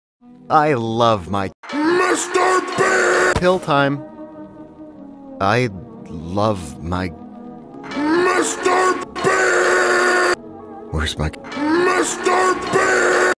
I love my MRBEAST!
Rip in pepperoni never forgetti

My 3rd and final "The Real MrBeast" meme.

Ignore the weird issues where assets from future or past scenes flash through the scene they are not supposed to appear in for a few milliseconds, Clipchamp as an editor is really bad at fixing this problem.